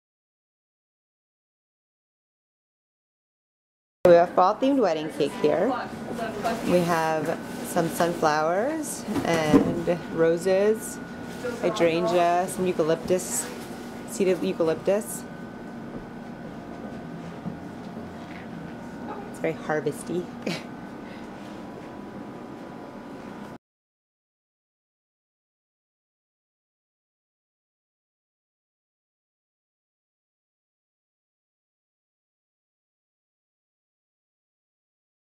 Sunflower Wedding Cake
We were happy to hear from this bride back in July when she asked us to do her wedding cake. Previously she had ordered this cake  from us for her cousin's party.

For her wedding cake, we created a rustic/fall theme with colors to match the color pattern of the wedding/invitations. Sugar flowers included sunflowers, black magic roses, circus roses, green hydrangea and seeded eucalyptus.